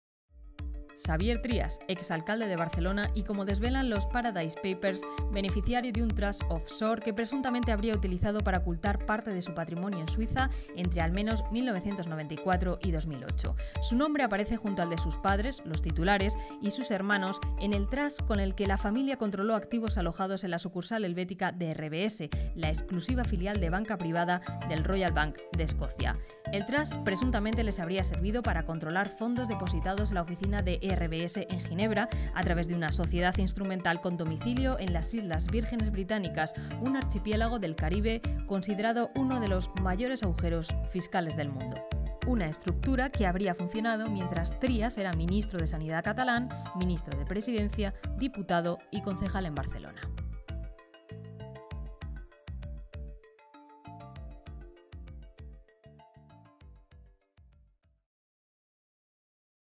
Papeles del Paraíso | Xavier Trias participó como beneficiario en un trust offshore
El exalcalde de Barcelona y actual portavoz del PDeCAT en el consistorio figura junto a sus padres y hermanos en el fideicomiso The JTB Family Settlement, un vehículo para ocultar activos

El pasado viernes, en una entrevista realizada en el Parlament de Catalunya por La Sexta y El Confidencial, el exalcalde de Barcelona aseguró que desconocía la existencia de esta estructura offshore. “No tengo ni idea porque nunca me he cuidado de esto. Si esto es verdad, no tengo ni idea”.

¿Qué son los Paradise Papers? Dos despachos de abogados, 19 registros mercantiles, 13,4 millones de documentos, 1,4 terabytes de información. Una nueva filtración obtenida por el diario alemán Süddeutsche Zeitung y compartida con el Consorcio Internacional de Periodistas de Investigación (ICIJ) y otros 94 medios, entre ellos El Confidencial y La Sexta en España. Conoce todos los detalles de esta investigación mundial sobre los paraísos fiscales en este vídeo.